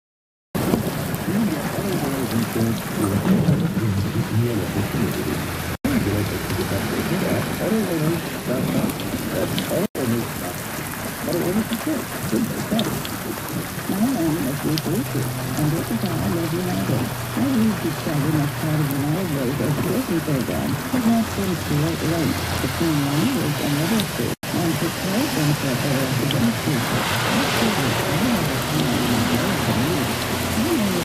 Boise 100-year Storm. 7/8/2015 by Robert Scott
Over an INCH of rain fell in a couple of hours! Weather forecater said it was a 100-year- storm. My Son, David and I were out at Lucky Peak reservoir, just getting to dive in off the dock, when we were hit by this " Biblical" storm! We could SEE the waves of rain coming at us from across the lake! It was like being hit with bucket after bucket of water. VERY REFRESHING!